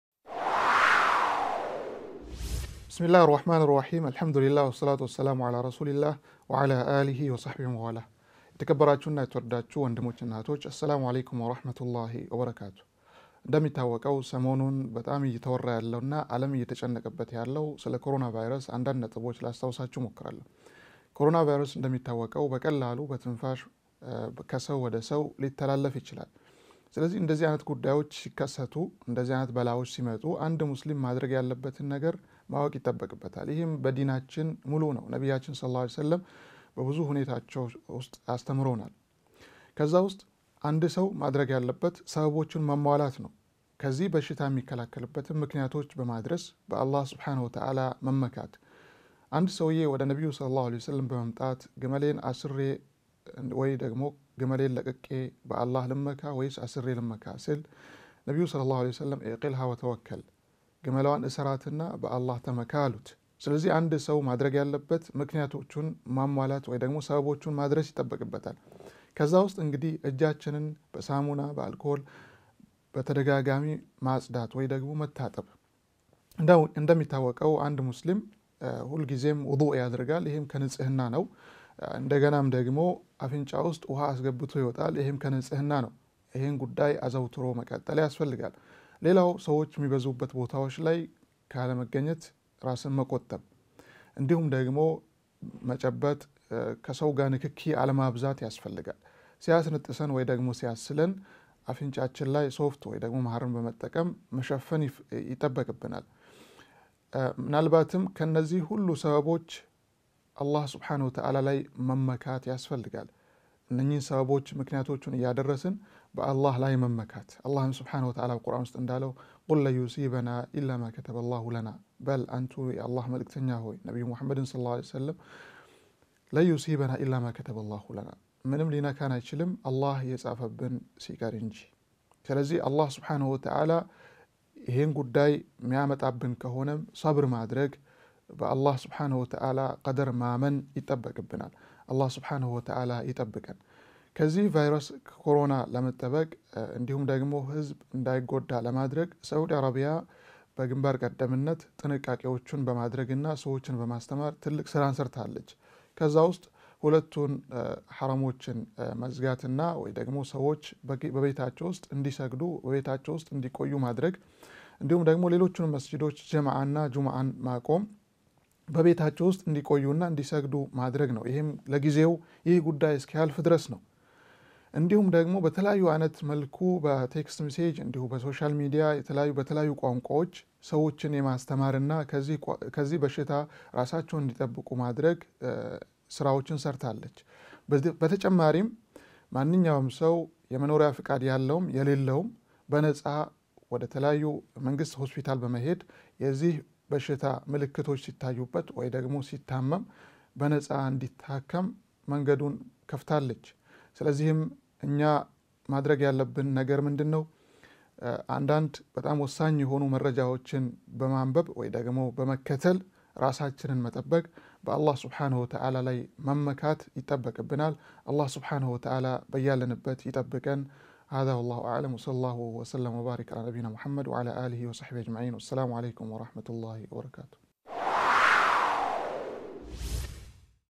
فايورس_كورونا تنبيهات وتوجيهات باللغة الأمهرية
የ#ኮሮና በሽታ ነፃ ሕክምና እና ራሳችን ለመጠበቅ የሚረዱ ነጥቦች.